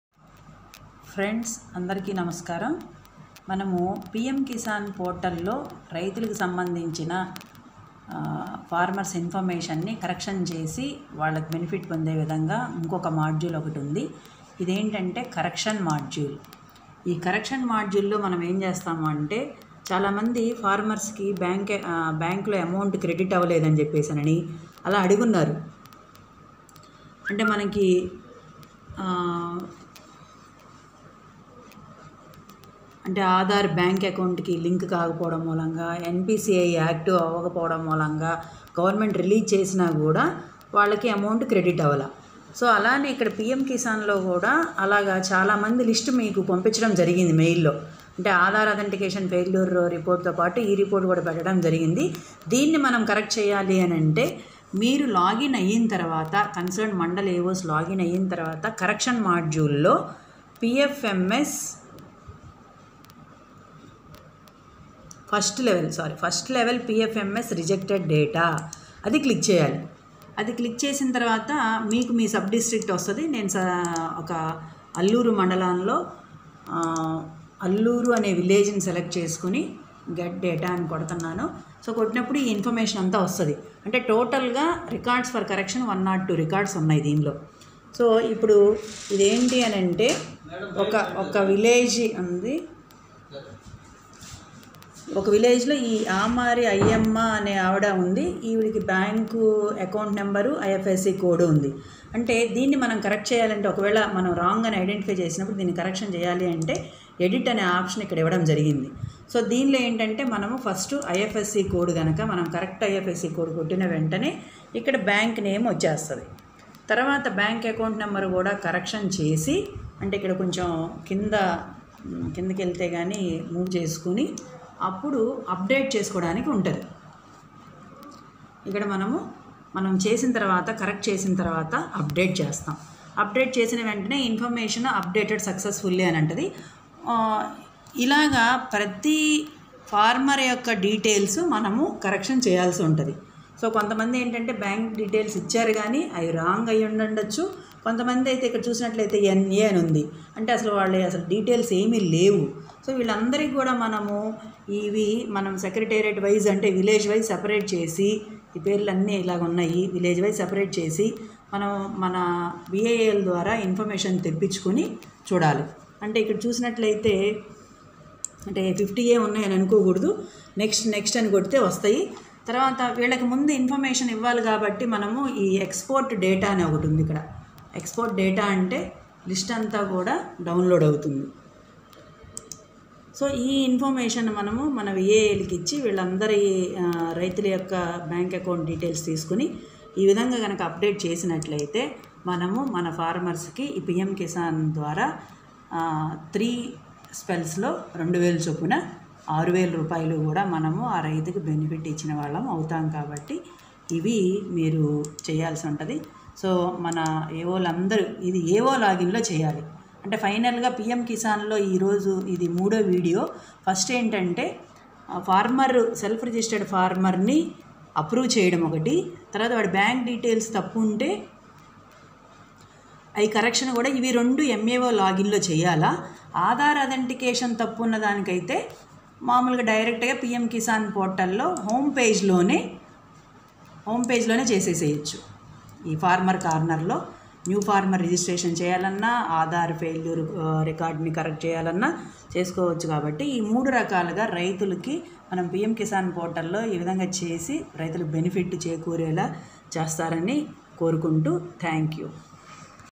ప్రధానమంత్రి కిసాన్ యోజన డబ్బులు పడలేదా అయితే ఈ వీడియో చూడండి
Pm kisan correction module procedure video